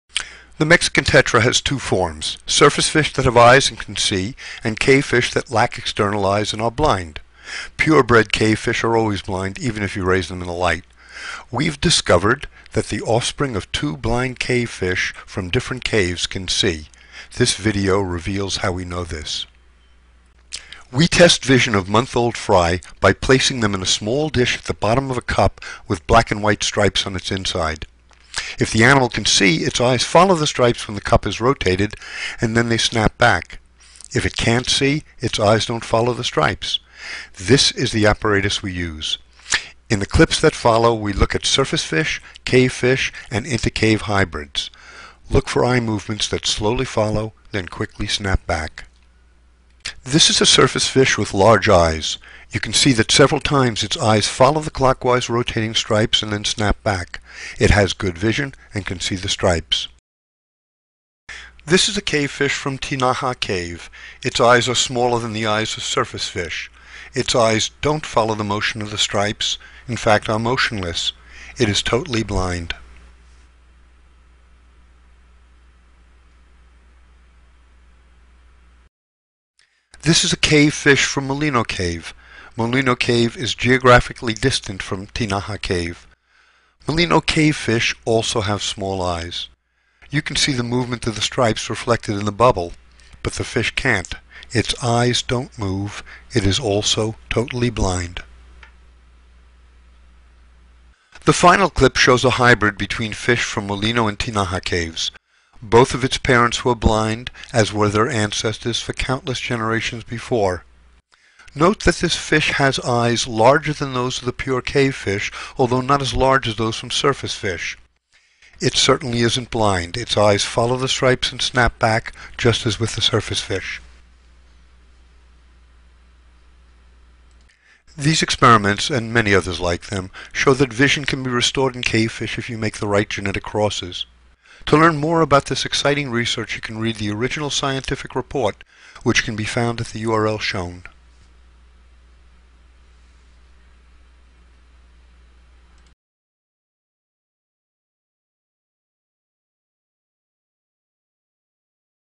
De blinde holbewoner - Restoring Vision to Blind Cave Fish
De gitzwarte grotten in het Mexicaanse stroomgebied van de Rio Grande vormen de thuisbasis voor de Astyanax mexicanus, onder gewonere mensen bekend als de blinde holenvis of grottenzalm. Twee lichaamskenmerken van deze donkerspecialist schitteren door afwezigheid: de grotvissen hebben geen kleur en geen ogen. Lees meer in het boek 'De stinkende scharrel papegaai'